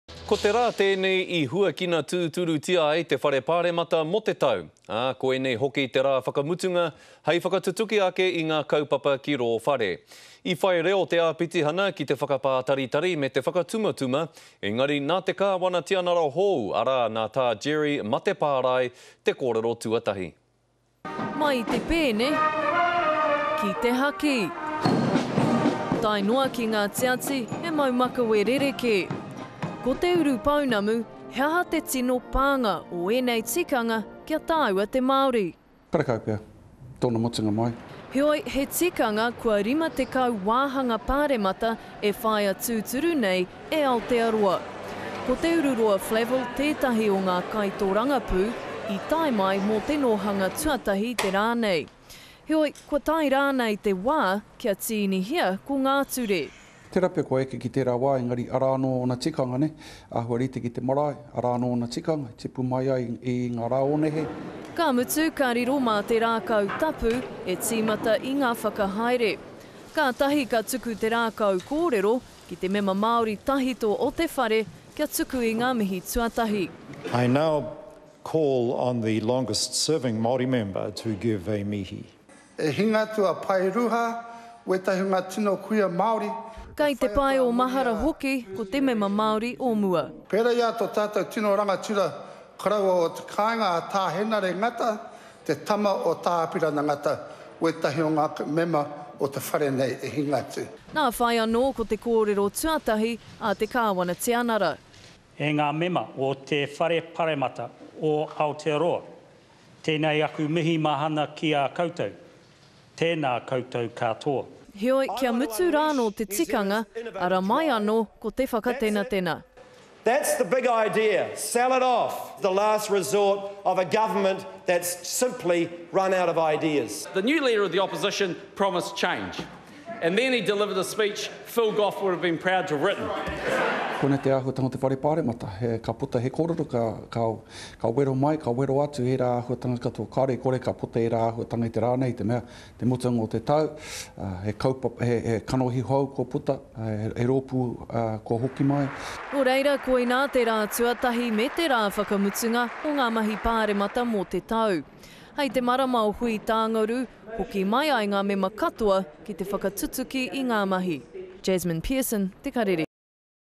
Parliament officially opens
Ko te raa teenei e huakina tuuturutia ai te Whare Paaremata moo te tau - aa koinei hoki te raa whakamutunga hei whakatutuki ake i nga kaupapa ki roo whare. I whai reo te aapitihana ki te whakapataritari me te whakatumatuma, engari, na te Kaawana Tianara hou, na taa Jerry Matepaarae te koorero tuatahi